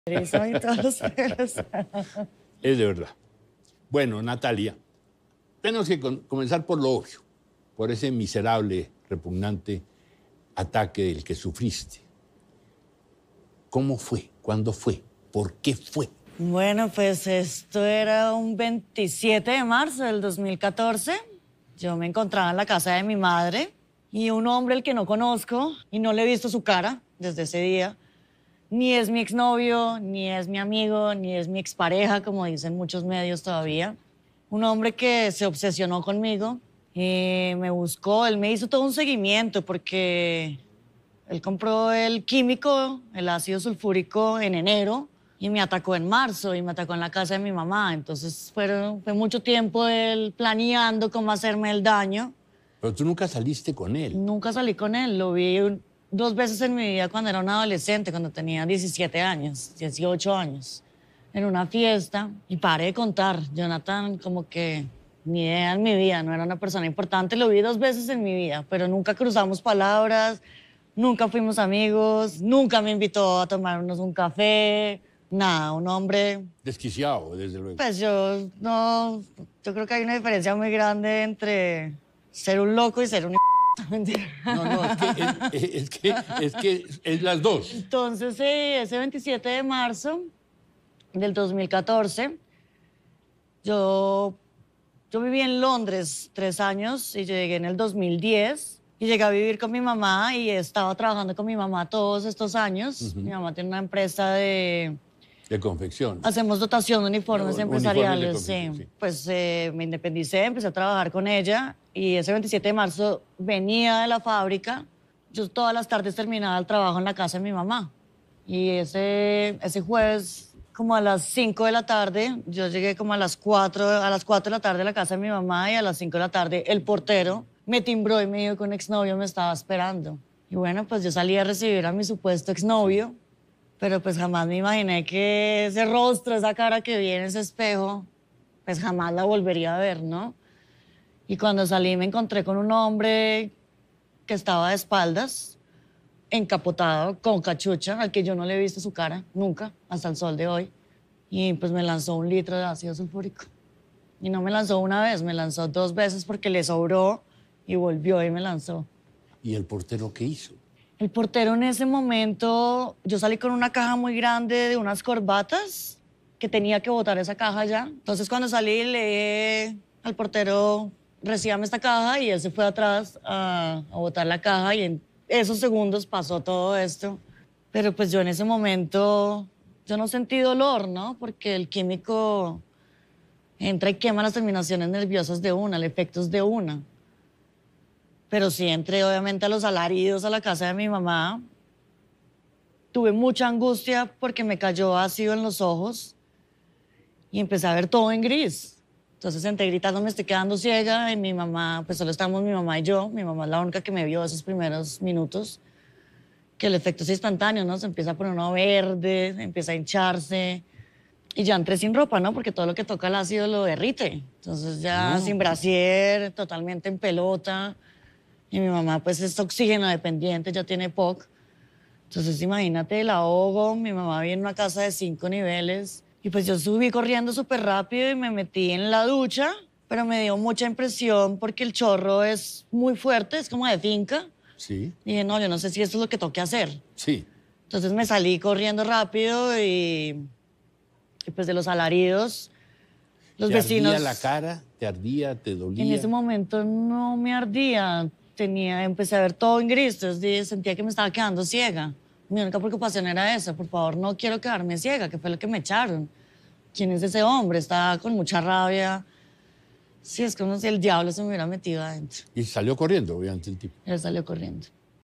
Natalia Ponce de León recordó lo que pasó aquél día que su vida cambió para siempre
Natalia Ponce de León conversó con José Gabriel, y aunque muchos ya saben su historia, ella recordó parte de lo que vivió el pasado 27 de marzo de 2014.

“Un hombre se obsesionó conmigo, me buscó, me hizo un seguimiento, me atacó en marzo en casa de mi mamá. Nunca salí con él, lo vi dos veces en mi vida, pero nunca cruzamos palabras, nunca fuimos amigos, nunca me invitó a tomarnos un café”.

Natalia Ponce de León recordó lo que pasó aquél día que cambió su vida para siempre